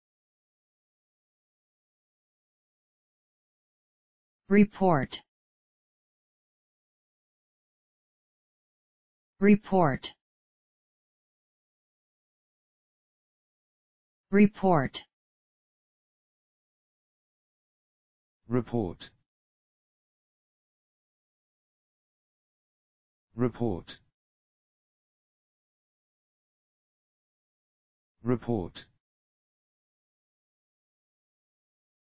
How to Pronounce reported in American English and British Englishreported
Learn how to say reported with HowToPronounce Free Pronunciation Tutorials.

This video shows you how to pronounce reported correctly in American English and British English.